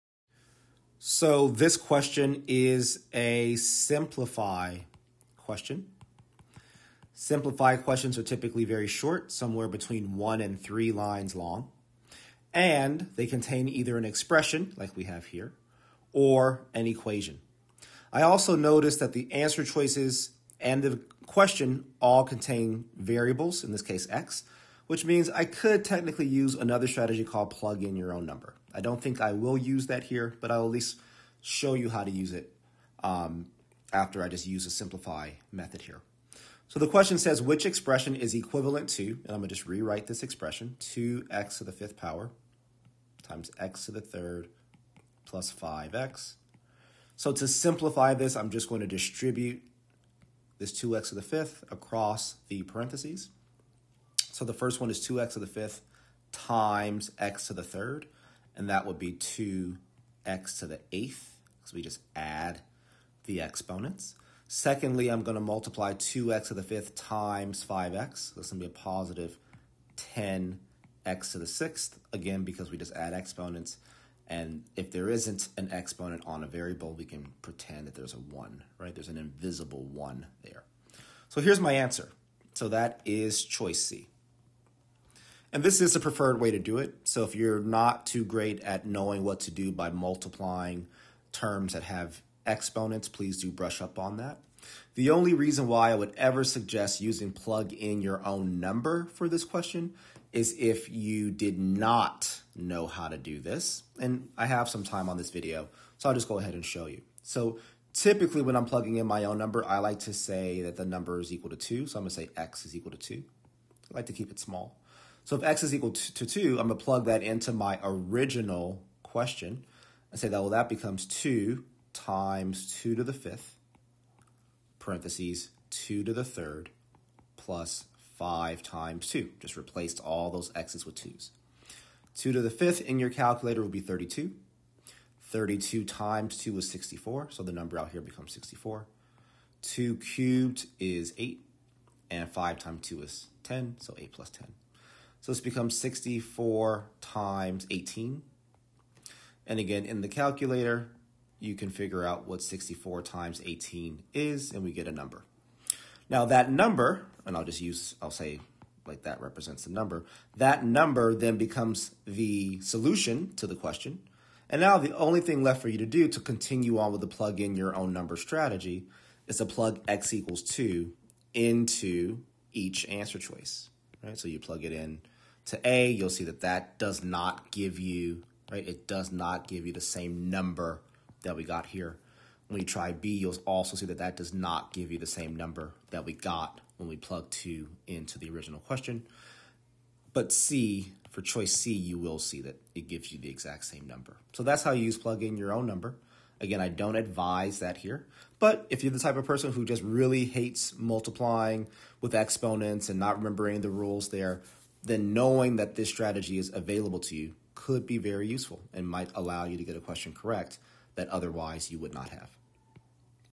Which expression is equivalent to 2x^5(x^3 + 5x) ?
Official May 2022 International SAT Section 4 Question 4